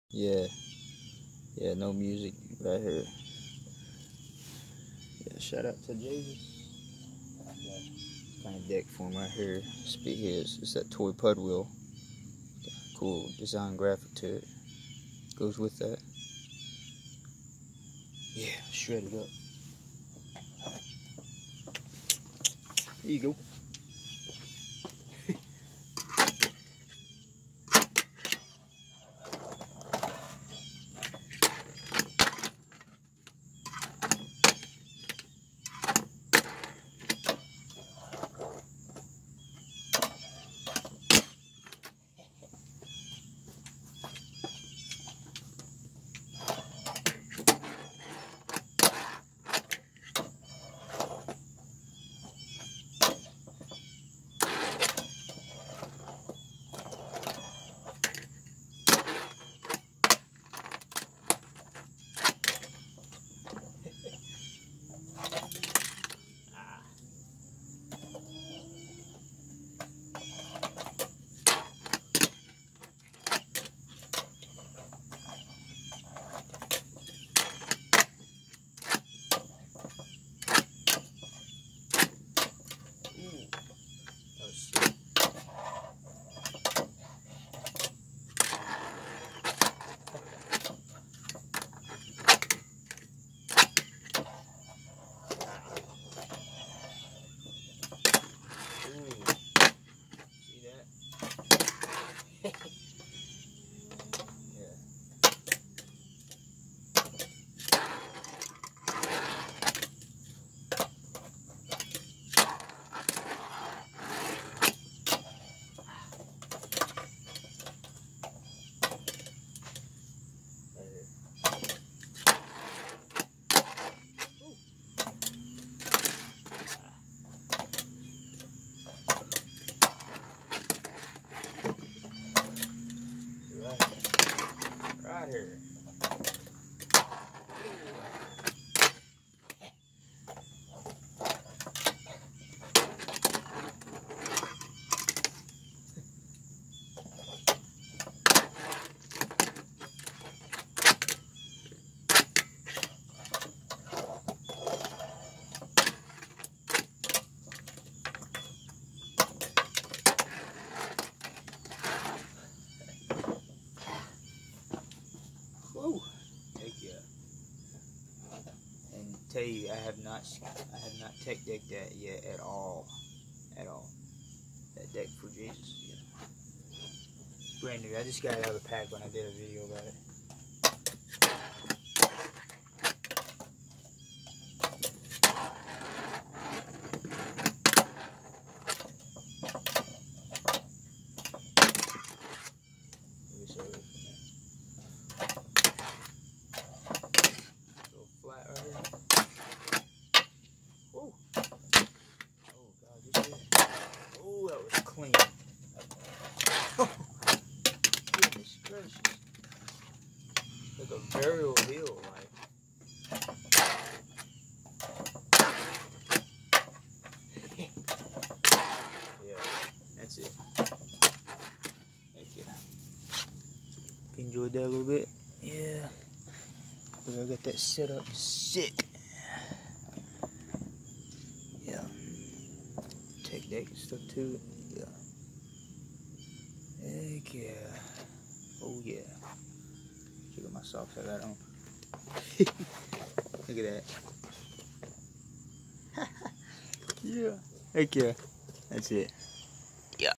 Sunny and cloudy early this morning while in tech decking😀
On my green tables out on my back porch tech decking my set up park😀